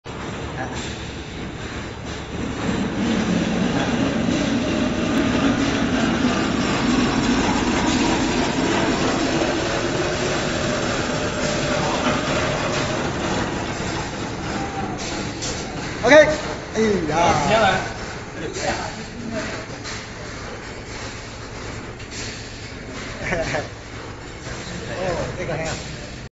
pumpkin wax gourd papaya peeler/fruit vegetable jackfruit grapefruit pomelo peel peeling machine
pumpkin wax gourd papaya peeler/fruit and vegetable jackfruit grapefruit pomelo peel skiving peeling machine

1.Stainless Steel Wax Gourd Peeling Machine peel pawpaw, pumpkin , Hami melon(dew melon), wax gourd(white gourd), and pomelo(grapefruit).
2.Its peeling thickness is adjustable from 1mm to 3mm.The cutter work circularly to peel upwards and downwards with high efficiency.
3.Peelingrate is about 98% melon skins. It applys to food and beverage industry, canteen, food industrial factory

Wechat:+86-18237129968
Skype ID: live:lisalymachine